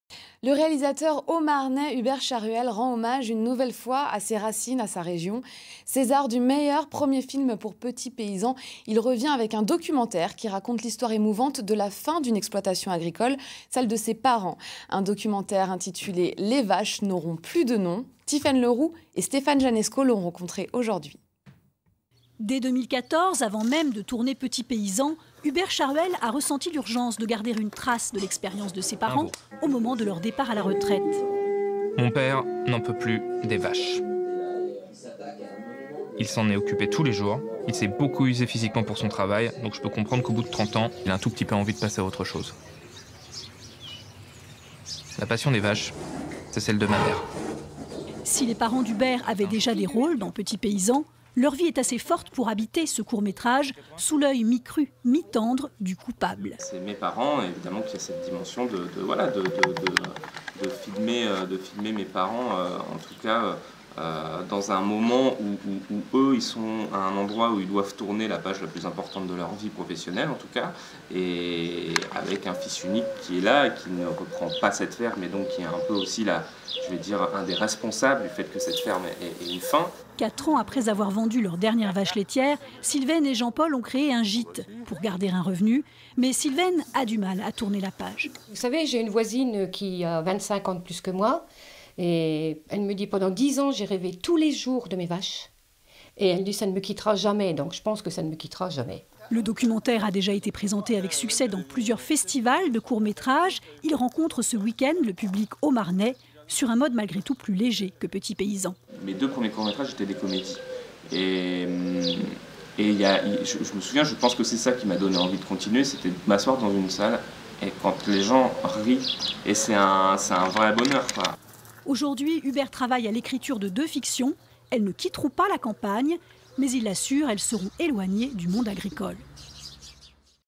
Documentaire de Hubert Charuel : "les vaches n'auront plus de nom"
Il avait emporté le césar du meilleur premier film avec "Petit Paysan". Le Haut-Marnais Hubert Charuel creuse de nouveau son sillon agricole avec un documentaire diffusé lundi  lundi 2 mars à 23h30 sur France 3 Grand Est. "Les vaches n'auront plus de nom" racontent la fin de l'exploitation agricole de ses parents. Tiphaine Le Roux et Stéphane Janeczko ont rencontré le réalisateur de ce court métrage de 50 minutes à Droyes son village d'enfance.